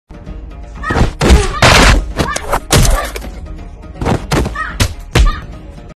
Fugimania🔥KUNG FU (Kouda Hetong)
Fugimania Kung Fu sparring with his daughter on film.